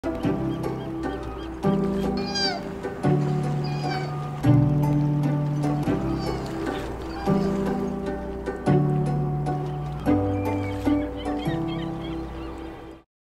Volume up to hear this Sea Otter Pup squeal!
Watch until the end to see this Sea Otter Pup munching on a crab, and volume up to hear its cute squeals!

While walking on the Old Fisherman's Wharf (in Monterey, California), I heard a sound that I’d never heard before. I looked over the edge, down to the water, and was surprised to see this young sea otter pup swimming around and squealing. I barely got my camera up in time to record it for a few quick seconds before its mom showed up with a crab. Such a cute moment with a happy ending.

Unfortunately, since I was in a hurry to film (and record the sounds of the pup’s squeals) this resulted in shaky video. Also, the light couldn’t have been much worse. This was early in the morning and mom & pup were backlit, with the sun rising behind them. Still thankful for this encounter!

This video footage is from 2021. I’m heading back up to Monterey, California soon and hope to have some more moments like this.

CAMERA EQUIPMENT USED:
Canon EOS R5 with Canon RF 100-500mm lens